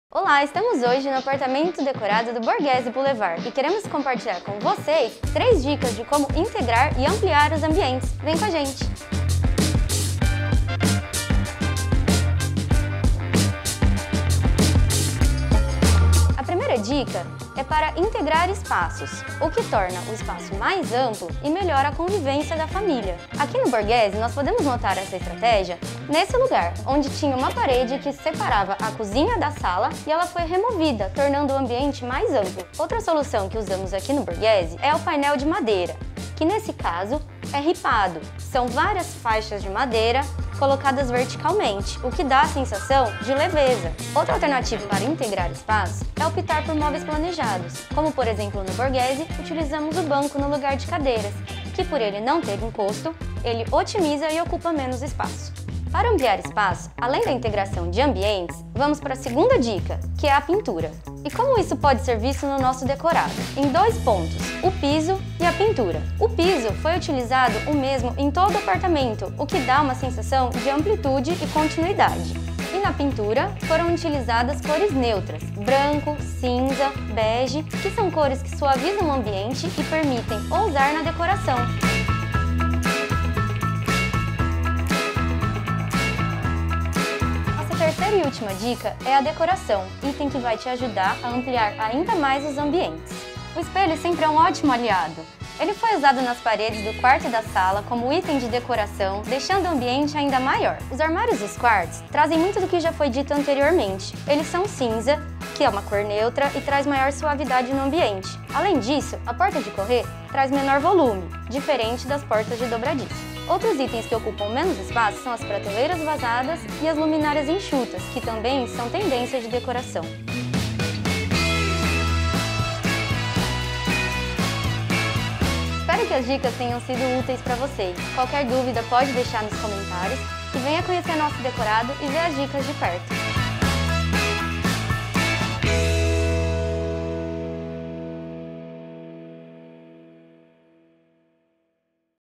3 DICAS PARA AMPLIAR O ESPAÇO DO APARTAMENTO | TAFLEX
Confira três dicas que a Julia Mazzoni, da área de Projetos da TARRAF/Taflex, preparou para você. Veja como integrar os ambientes e ampliar os espaços do seu apê, com cores e acabamentos em tendência no mercado. São recomendações fáceis que fazem toda a diferença nos espaços.